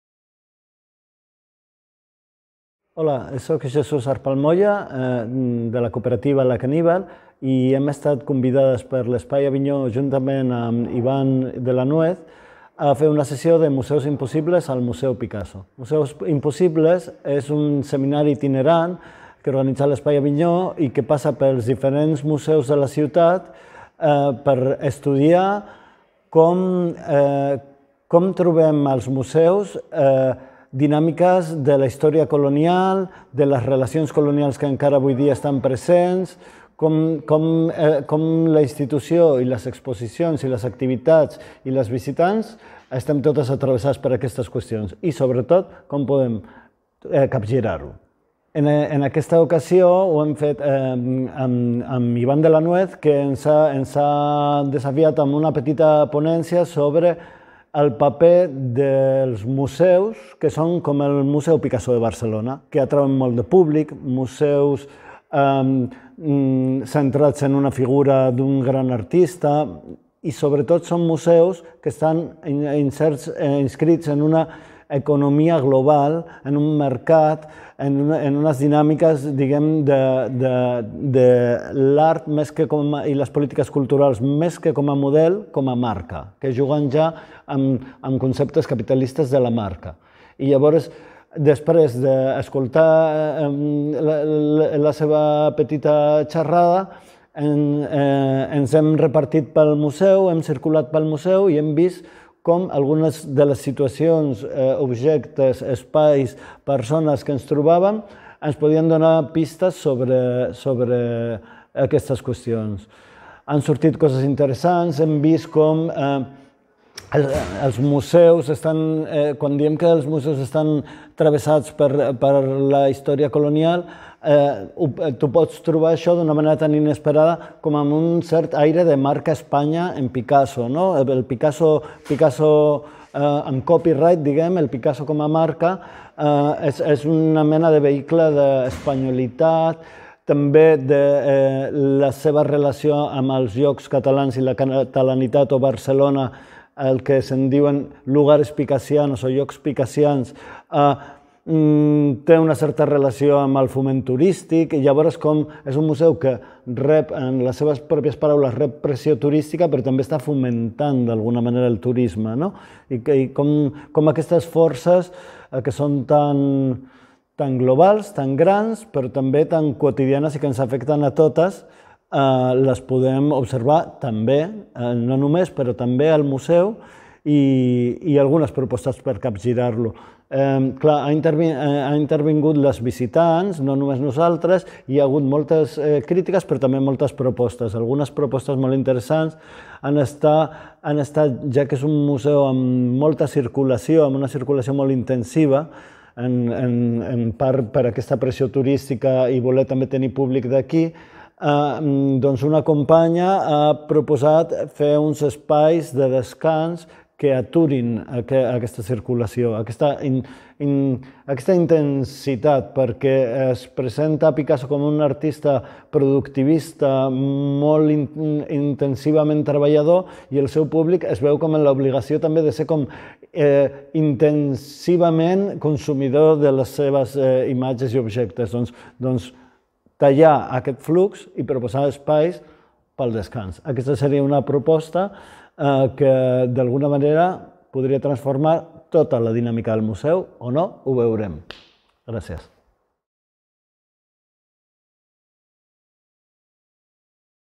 Museus (Im)possibles: «Entre la marca i el model»
Sessió de treball al voltant de la Col·lecció Picasso:
El Museu Picasso ha acollit aquest dijous 14 de març una de les jornades que conformen el cicle de Museus (IM)possibles que organitza l’Espai Avinyó de Barcelona sota el títol “Entre la Marca i el Model”. L’activitat, dirigida per l’assagista, crític i comissari, Iván de la Nuez, i l’artista Jesús Arpal-Moya, ha proposat un exercici de reflexions al voltant de la figura de Picasso i de les tensions entre els models de ciutat i del museu. En aquest sentit, els assistents de la jornada s’han plantejat qüestions com quines alternatives hi ha al model de museu actual? De museu democràtic? , a la marca de ciutat i als museus dominants o com es poden resignificar el llegat d'artistes consagrats en el món de l'art a través de processos educatius i de mediació dirigits a diversos públics  i que siguin processos crítics i transformadors.